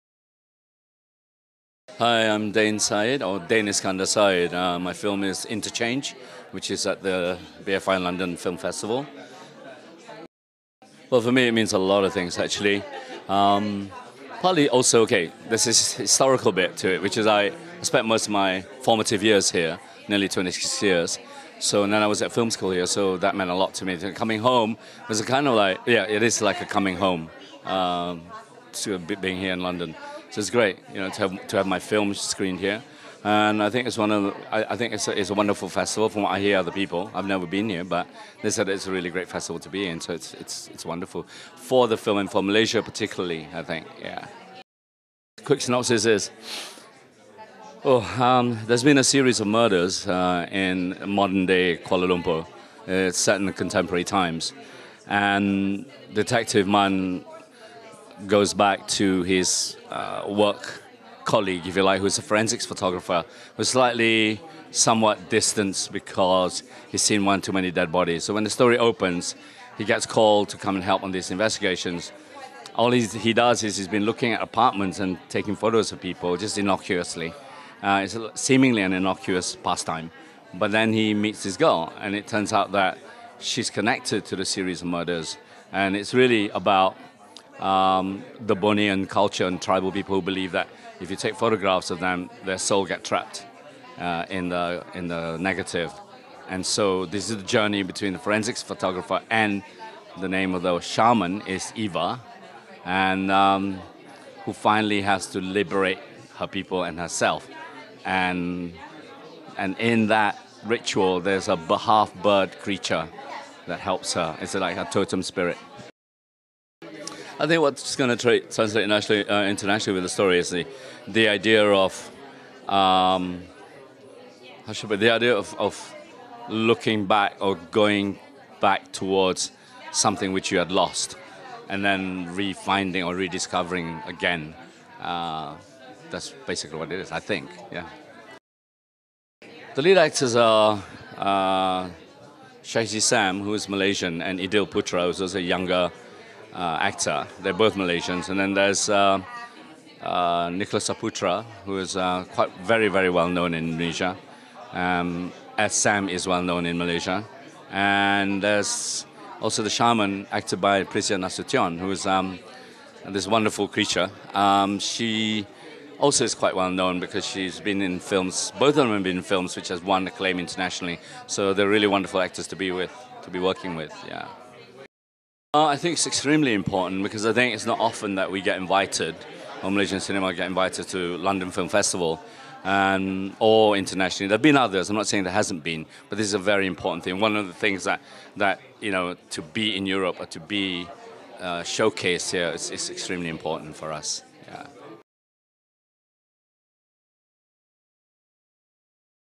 Interchange Director Dain Iskandar Said Interview
Red Carpet News TV talks to Interchange Director Dain Iskandar Said at the BFI London Film Festival 2016.